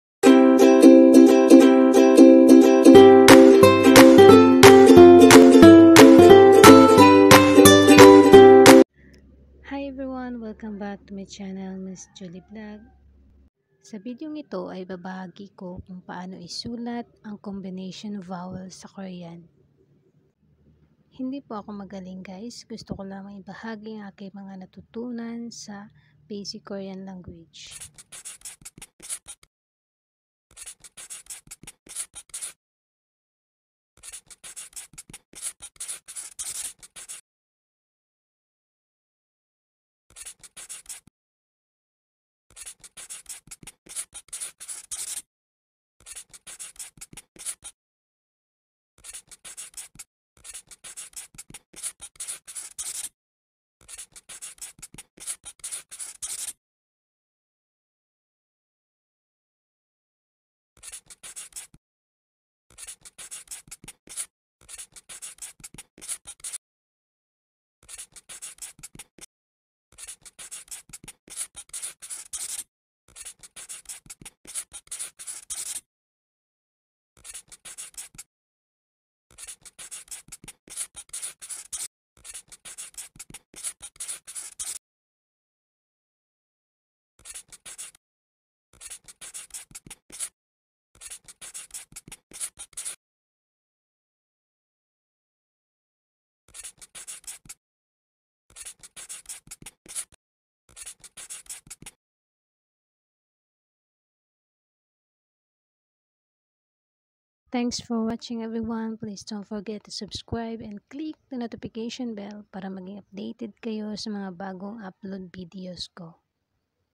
Combination Vowels in Korean | Ms Julie Vlog
Sa video a ito ay ibabahagi ko lang po kung paano isulat ang combination vowels sa Korean.